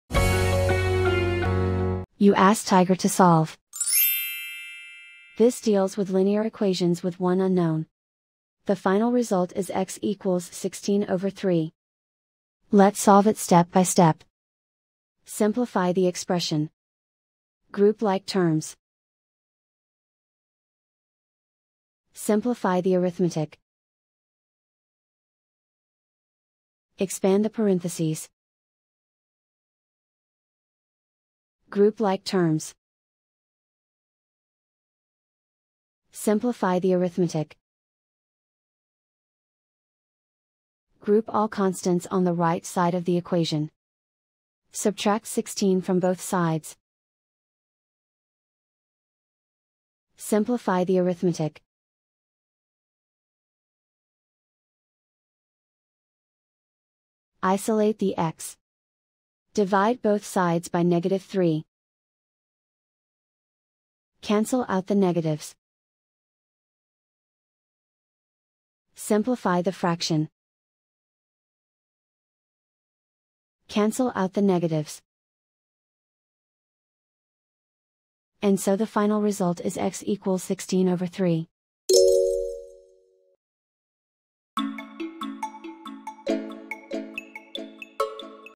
Solve 9-(6X+1)+3X+8=0: Linear Equation Video Solution | Tiger Algebra
Struggling with 9-(6X+1)+3X+8=0? Watch our step-by-step video on Tiger Algebra to master this linear equation effortlessly.